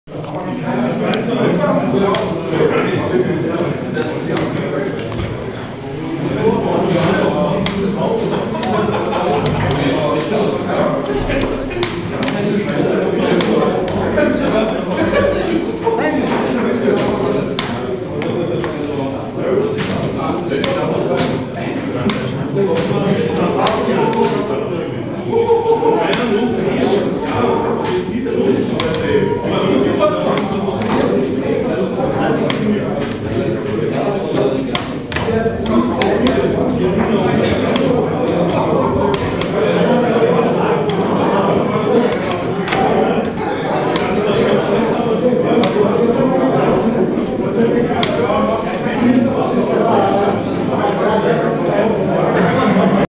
Frank Strobelt - Andreas Reiche
3. Himmelfahrtsblitzturnier der SG Weißensee für Mannschaften
17. Mai 2012 im Frei-Zeit-Haus Berlin-Weißensee
Viertelfinale, SG Weißensee 2 - SV Berolina Mitte, Brett 4